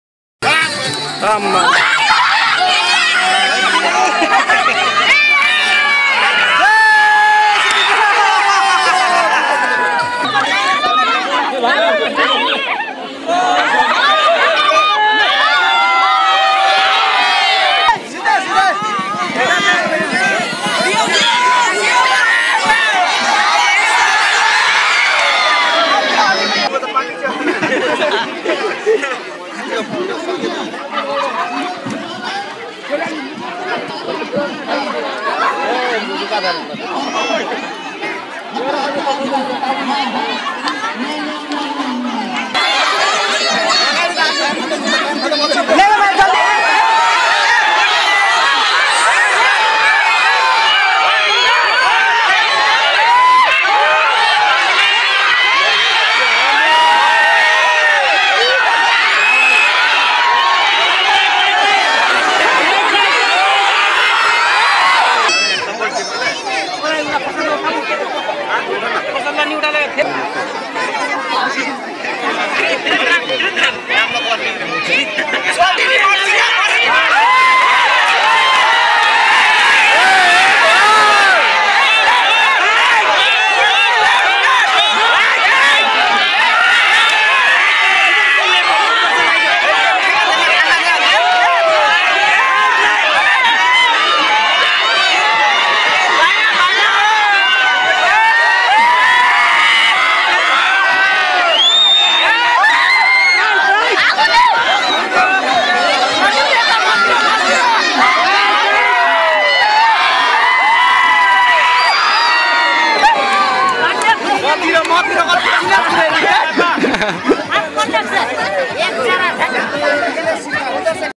Rajbanshi fun video (handi futta program during krishna janmastami )like video & subscribe channel
Rajbanshi film video devsari (handi futta program during krishna janmastami ) ghailadubba  2072 teknarayanbari